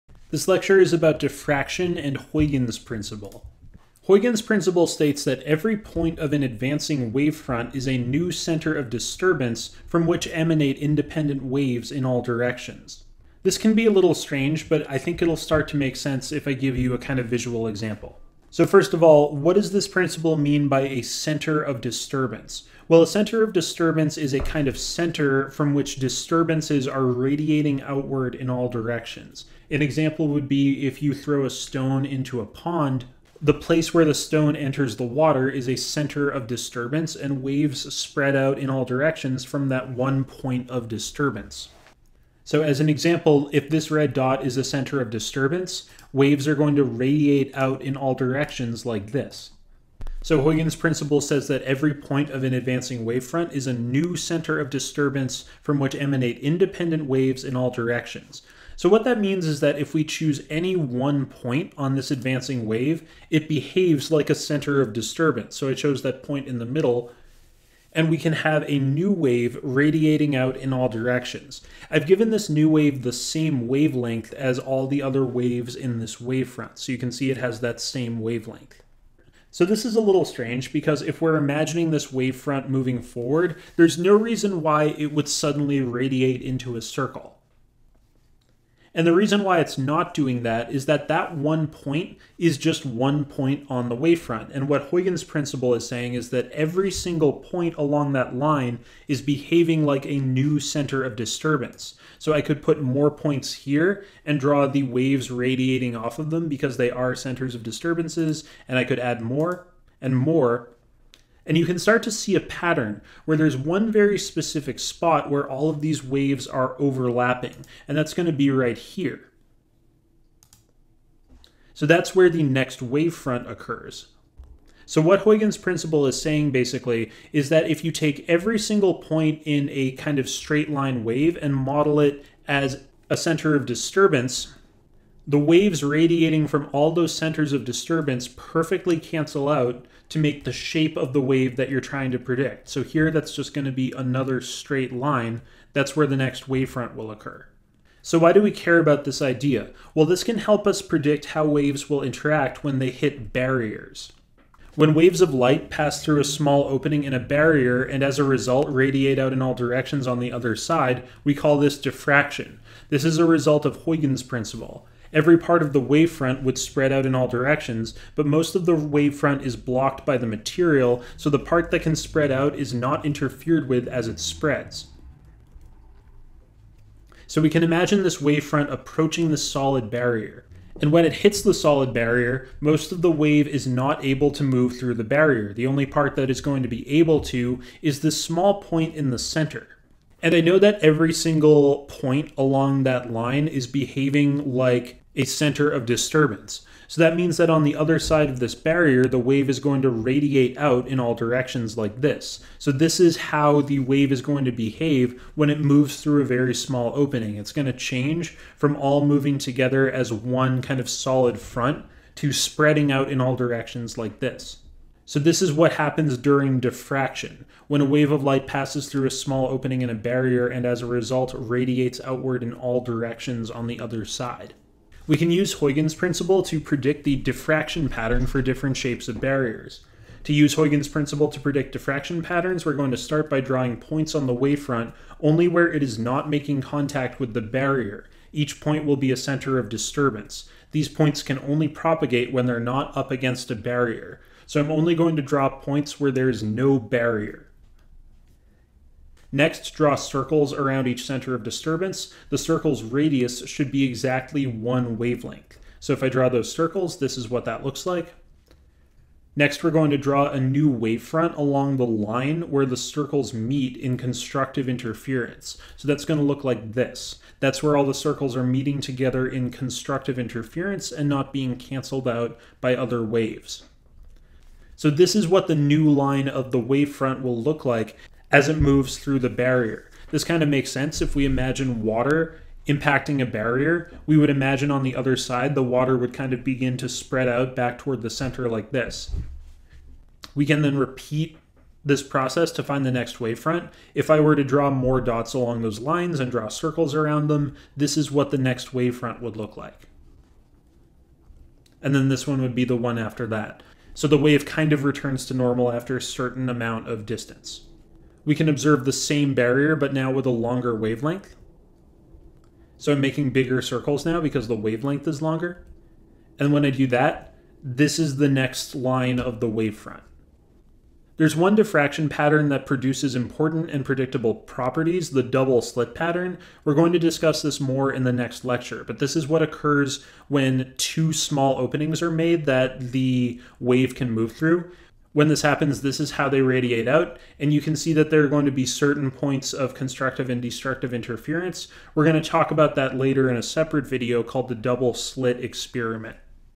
Diffraction and Huygens's Principle - IB Physics
NOTE: It's pronounced "Hi-gens" with a hard g. Don't embarrass yourself like I did!!!

I go over Huygen's Principle of Waves and how it's used to predict the shape of waves moving around barriers.

0:00 Intro to Huygen's Principle
0:22 What is a Center of Disturbance?
0:58 Visualizing the Principle
2:07 Summary of the Principle
2:27 Definition of Diffraction
3:45 Examples of Diffraction
5:20 Double Slit Diffraction Pattern